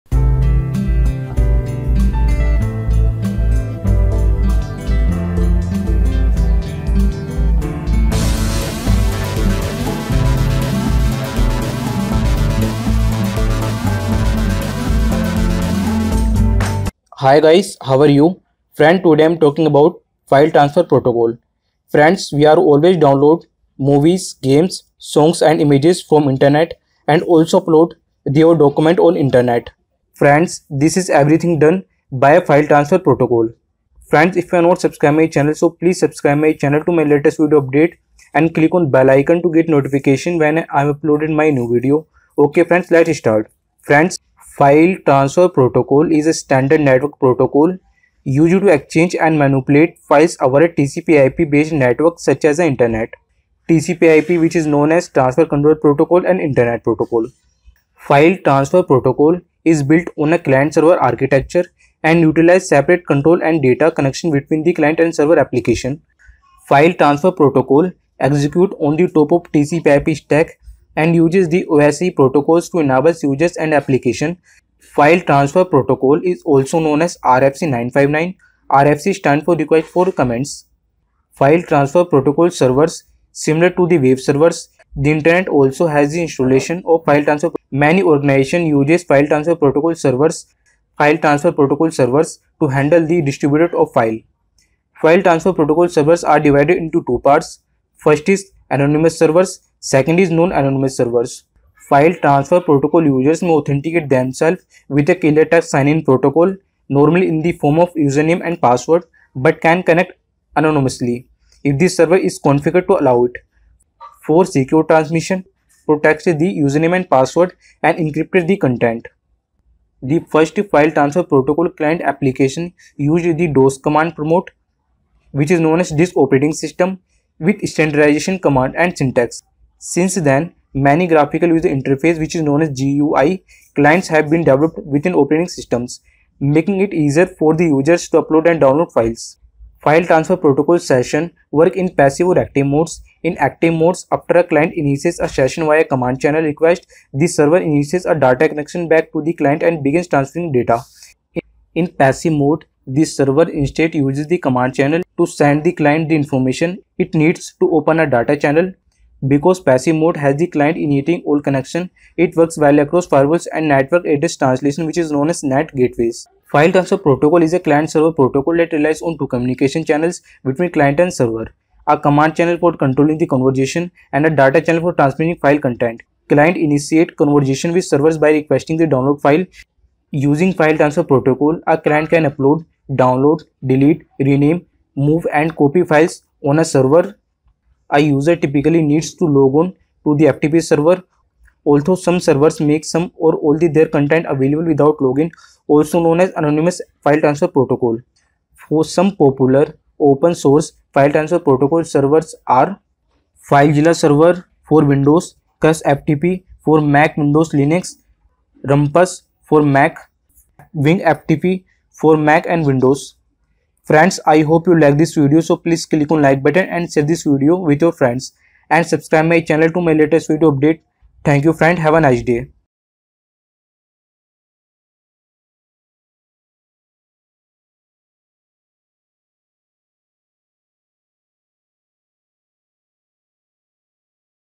File transfer protocol || how file transfer protocol work || FTP server
Hi guys how are you, friends in this video am  talking about File transfer protocol, Friends we are always download movies, games, songs and images from internet and also upload the our document on internet, friends this is everything done by file transfer protocol, i hope you like this video.